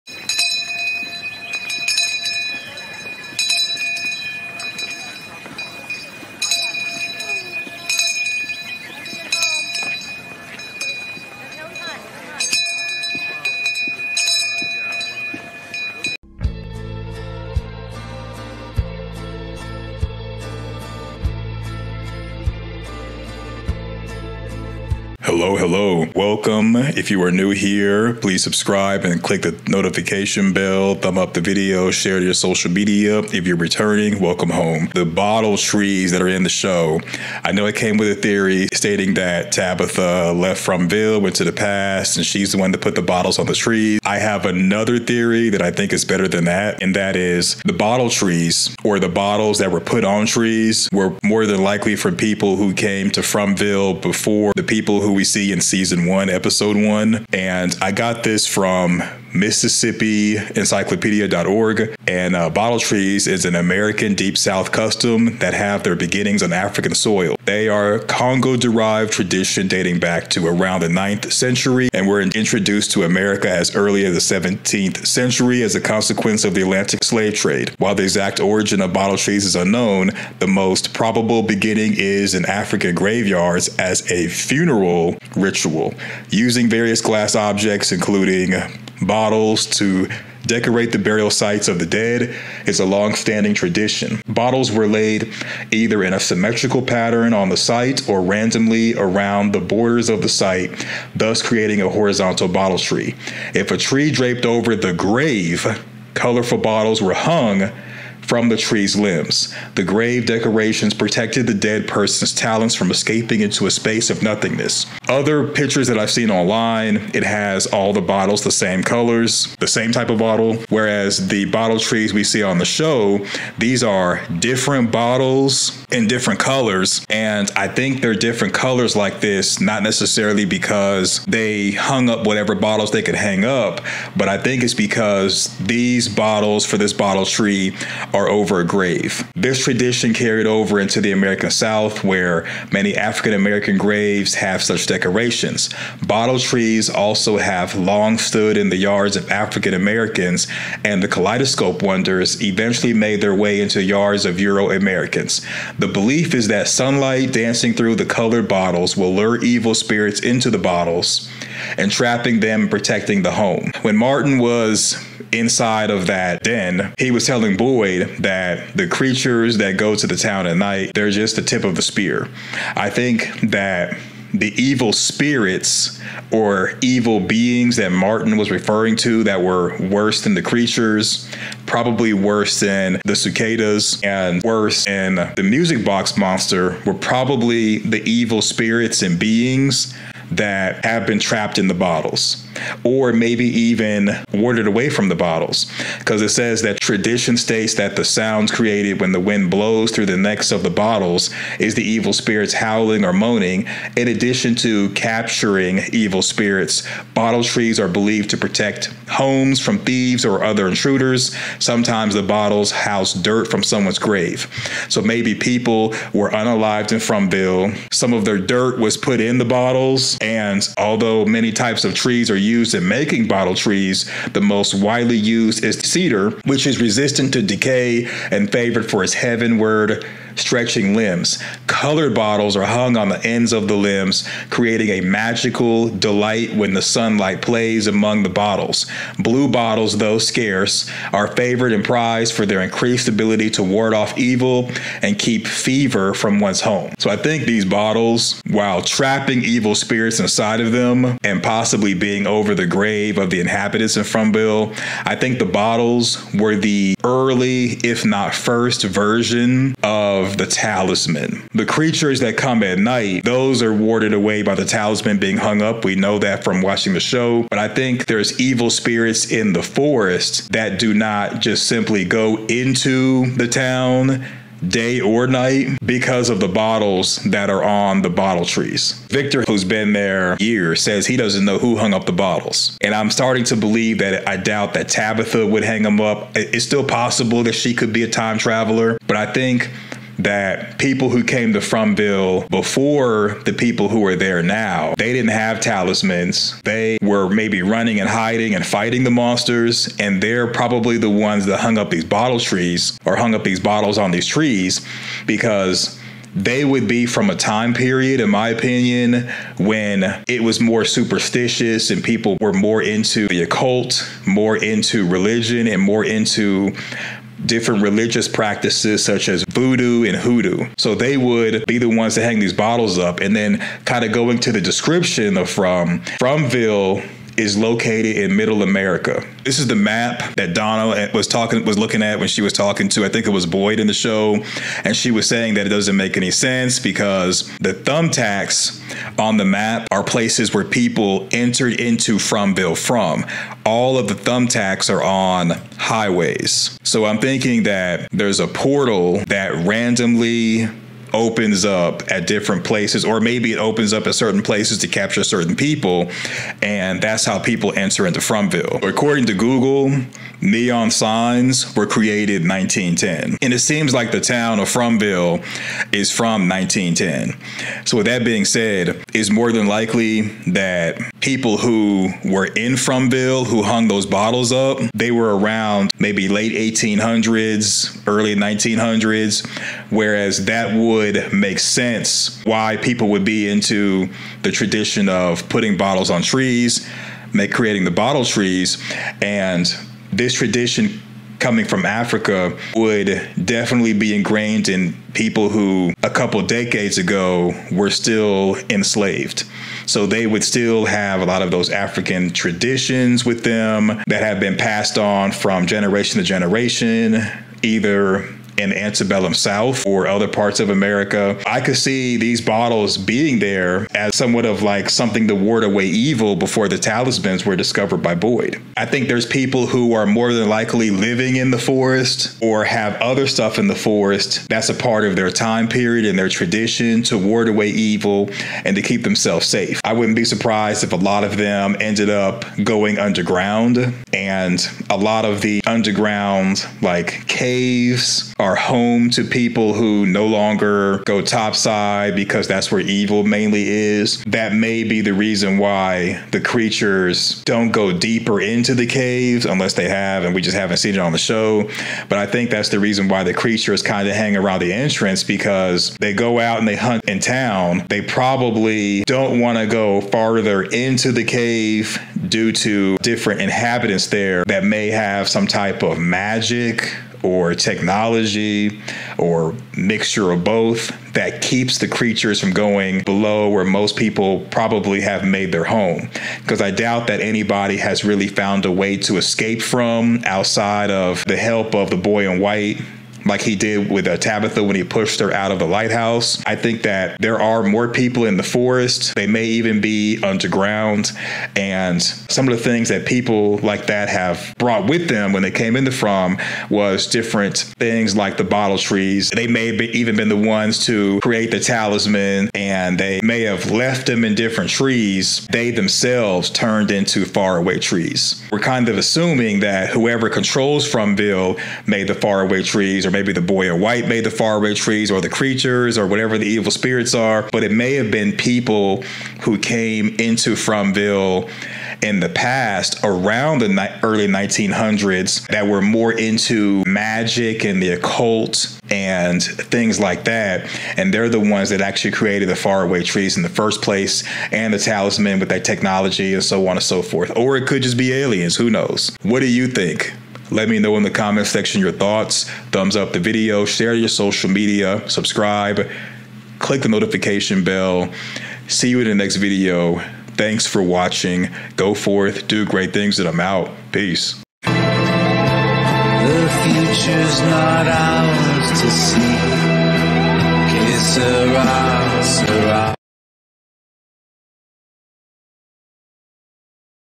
The Fromville Bottle Trees Were the Original Talisman Created by the First People | From
In this video we discuss the theories of the bottle trees being the original form to ward away evil before the talisman and possibly people inventing the faraway trees.